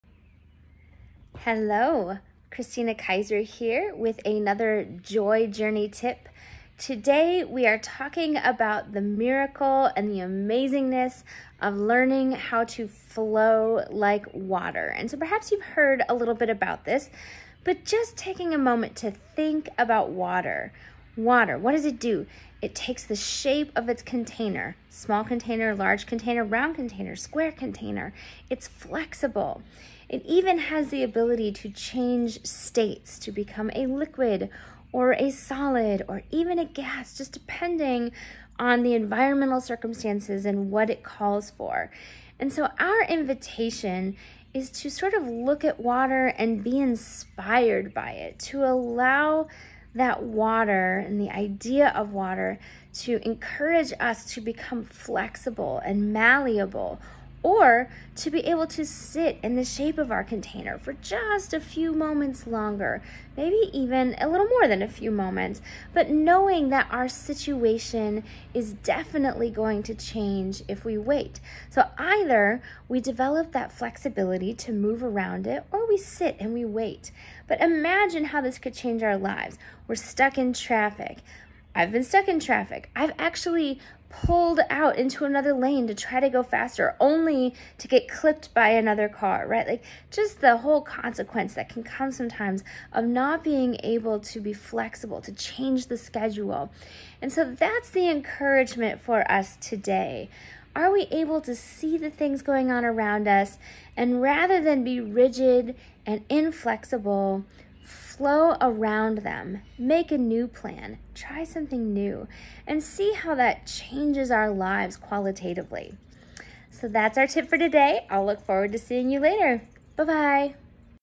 Flow Like Water
Water is a resilient thing, taking the shape of its container, flowing around obstacles and even changing states when necessary. When we are able to flow like water, life can become more joyful.